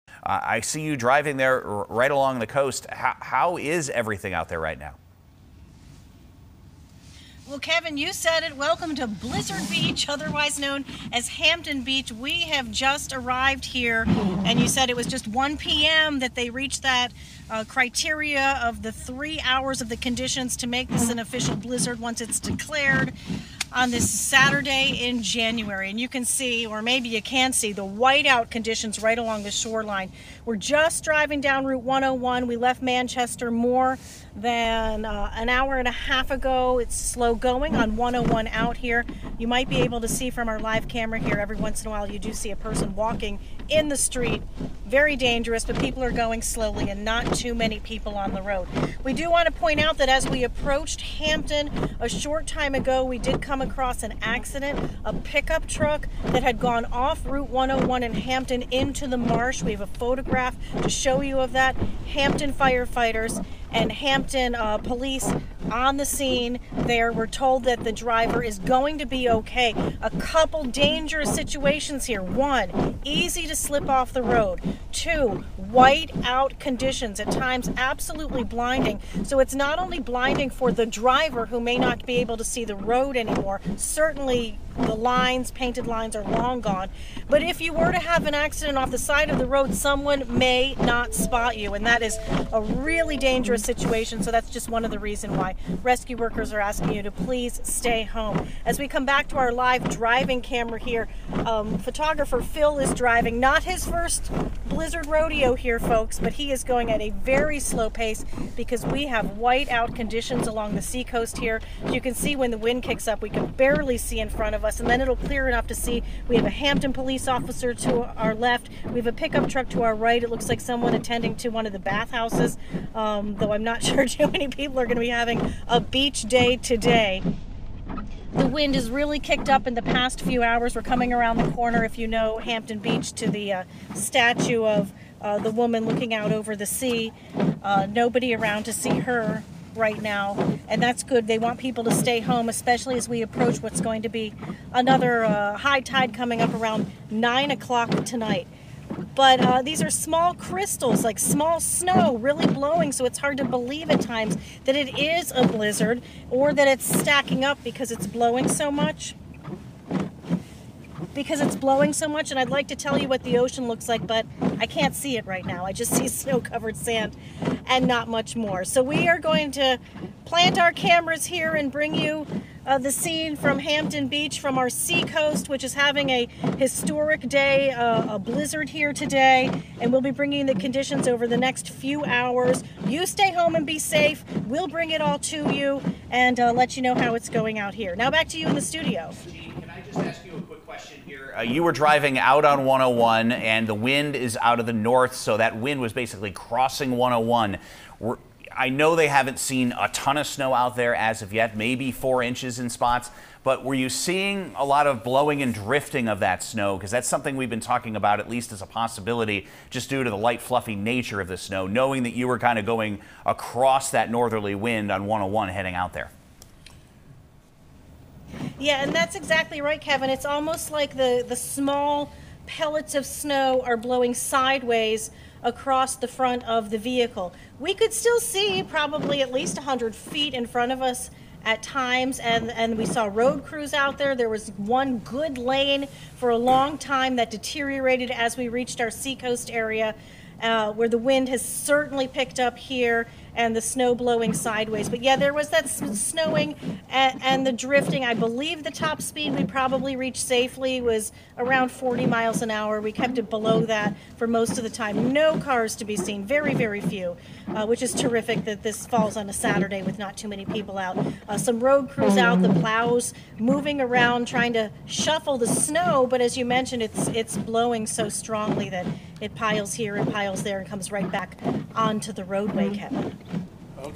Driving conditions a bit treacherous in Hampton
Jean Mackin shows just how driving is going in the Hampton area.